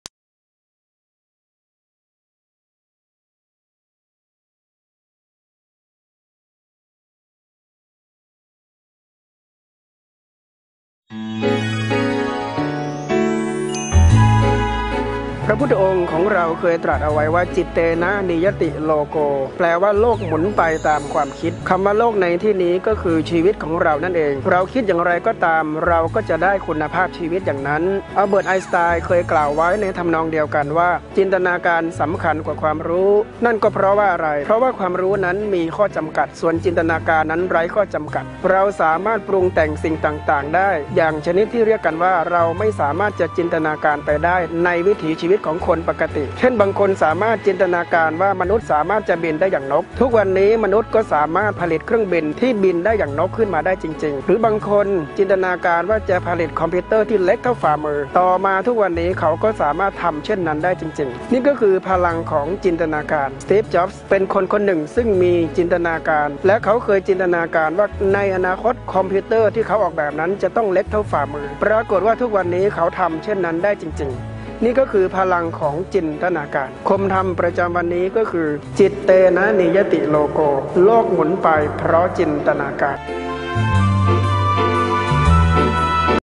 โลกหมุนไปเพราะจินตนาการ - พระมหาวุฒิชัย วชิรเมธี
พระมหาวุฒิชัย วชิรเมธี (ท่านว.วชิรเมธี) - สถาบันวิมุตตยาลัย
ทำดี.เน็ต ค่ายพุทธบุตรทำดี
ศูนย์วิปัสสนาไร่เชิญตะวัน ต.ห้วยสัก อ.เมือง จ.เชียงราย
Playlist เสรี ลพยิ้ม
Google Search  เสรี ลพยิ้ม    YouTube Search  เสรี ลพยิ้ม